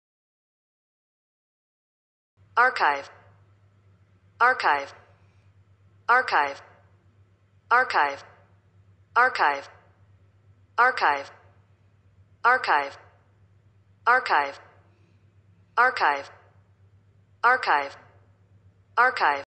How to pronounce Archive in english | Easy to learn pronunciation
In this short video we teach you ,how to
pronounce english word Archive properly.
You can watch this video and easily learn how to pronounce this word correctly.

any value in your life by teaching you right pronunciation.